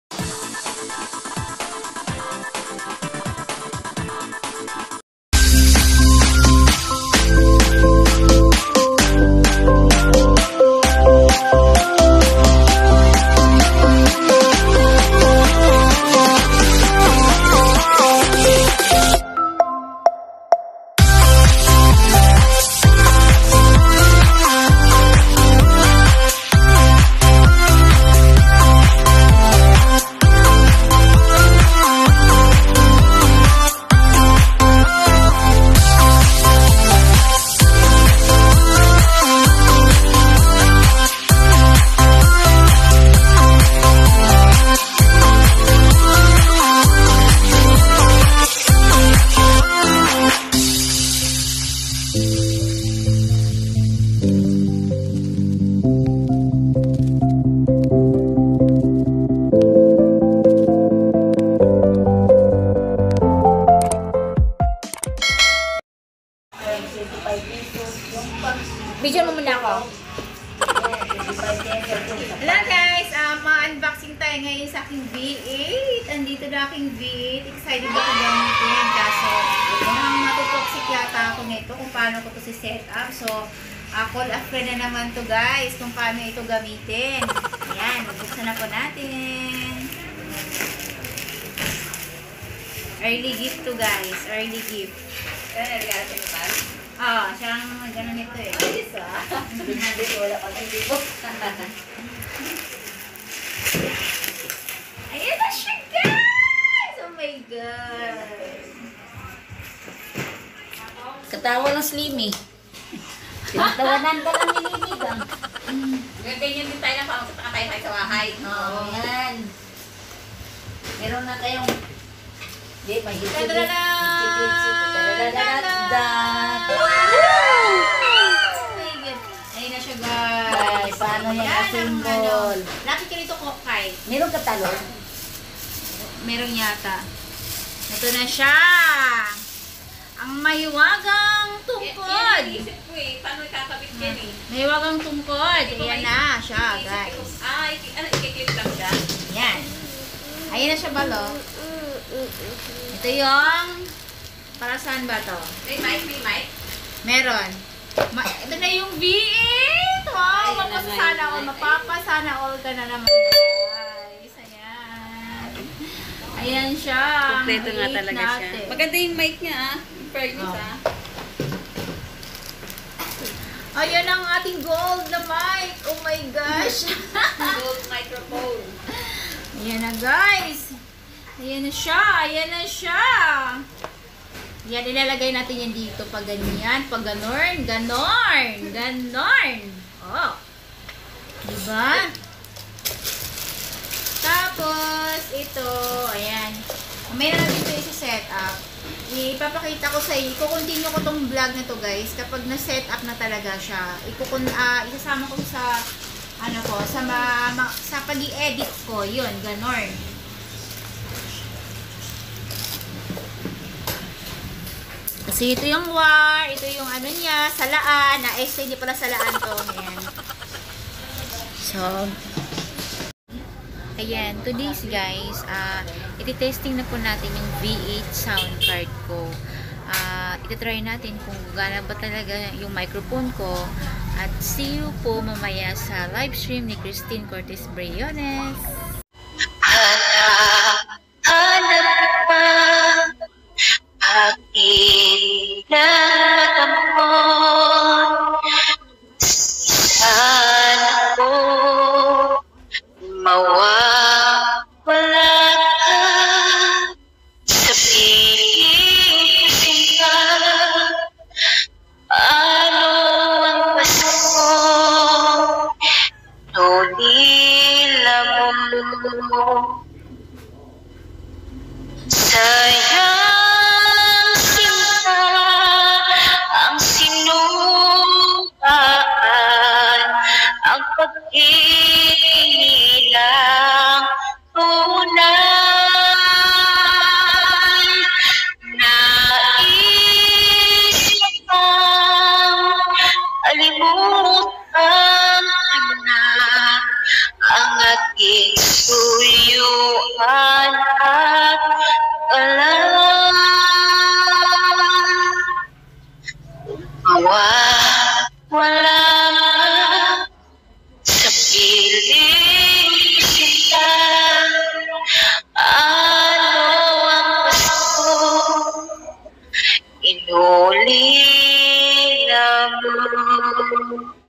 UNBOXING MY V8 SOUND CARD| ETHELY AND FAMILY | Gift from my enemy🤣
Samahan nyo guys buksan ang aking v8 sounds card regalo ng aking mortal enemy